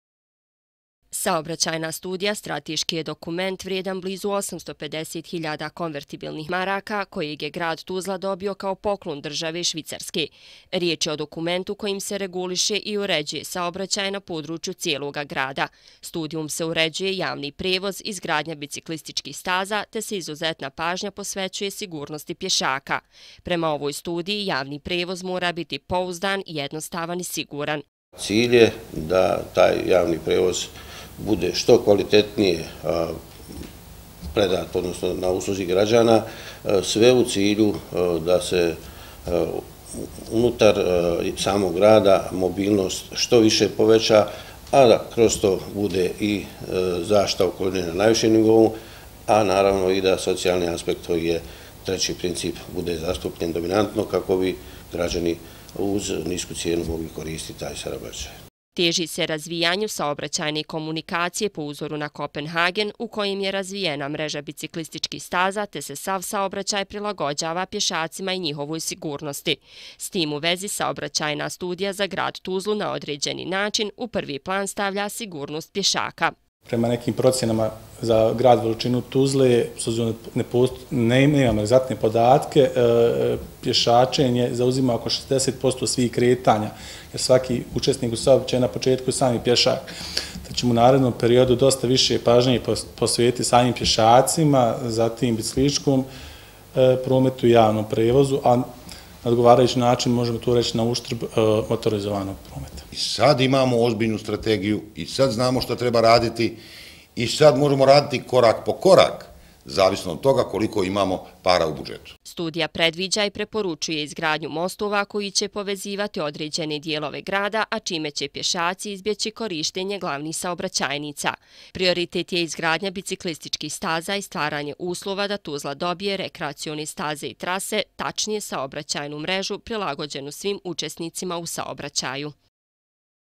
Predstavljena saobraćajna studija vrijedna oko 850.000 KM - 21.07.2015.
Na kolegiju gradonačelnika Tuzle danas je predstavljen Nacrt saobraćajno- prostornog koncepta grada Tuzla, odnosno saobraćajna studija koja za cilj ima regulisanje kompletnog saobraćaja na području grada. Studijom se uređuje javni prevoz, a istovremeno je akcentirana i izgradnja biciklističkih staza te sigurnost pješaka.